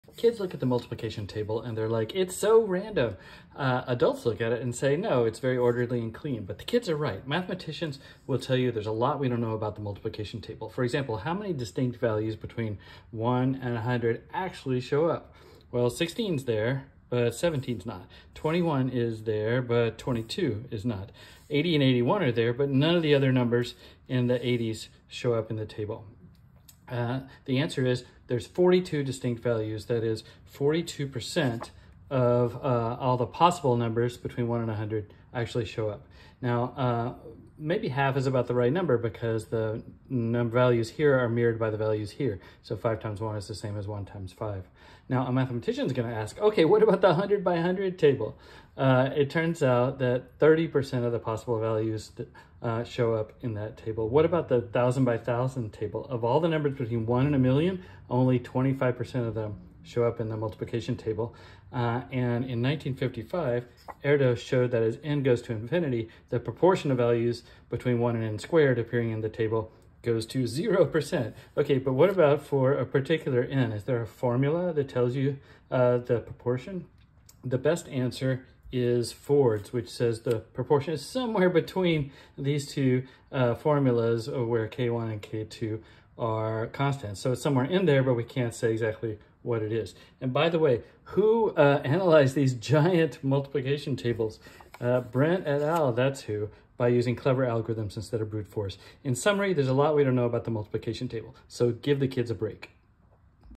The answer is 42?!
Every kid knows the multiplication table is pretty weird. For example, how many distinct numbers show up? 16 and 21 are there, but 17 and 22 are not. The answer is 42 of the possible numbers between 1 and 100 show up, or 42%. As n increases, that percentage seems to shrink. There's a lot of mystery still there...